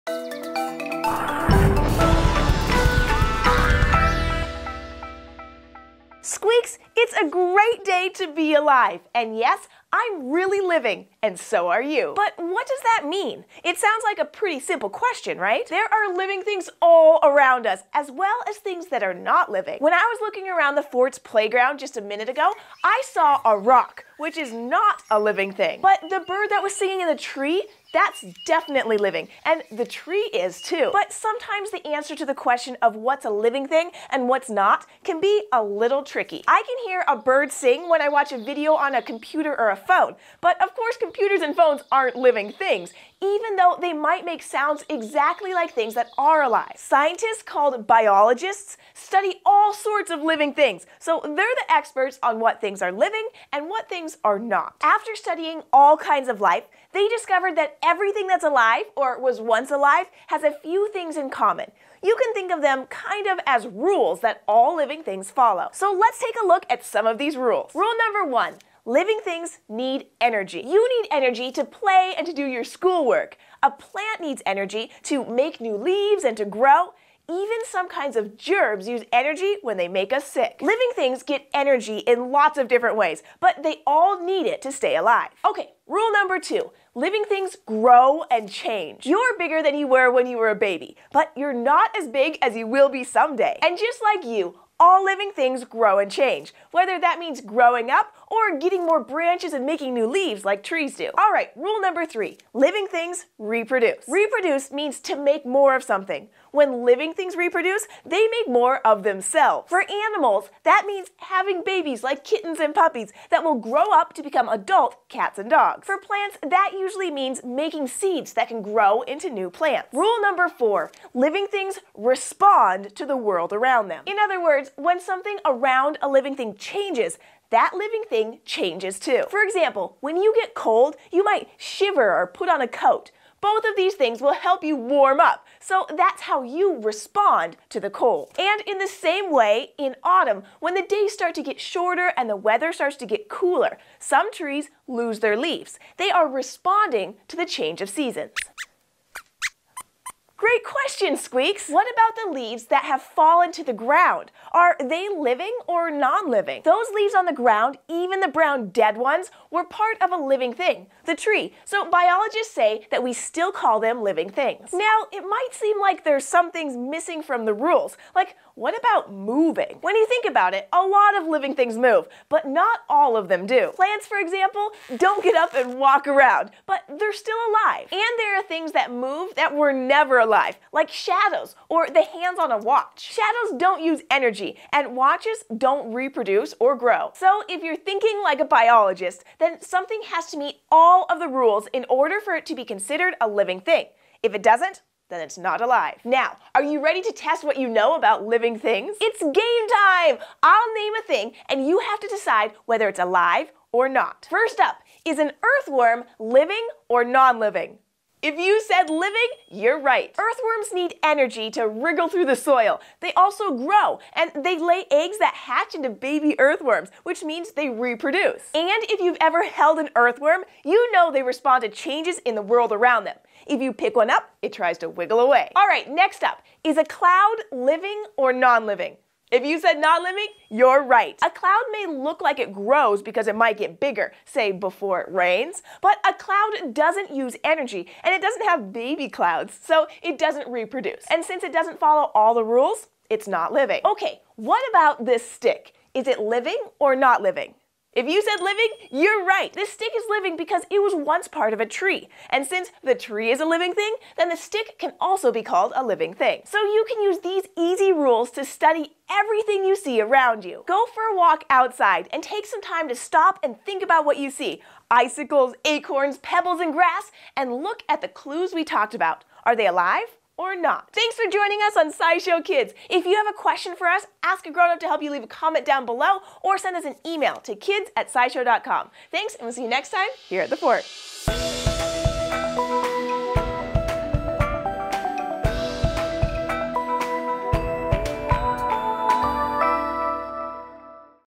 What Makes Something "Alive"? | Biology for Kids
You're alive! And so are all the plants in your garden, the birds in the sky, and the fish in the ocean! But how do we know they're alive? What does it even mean to be alive in the first place!? Join Jessi and Squeaks to learn how scientists decide if something is living or non-living!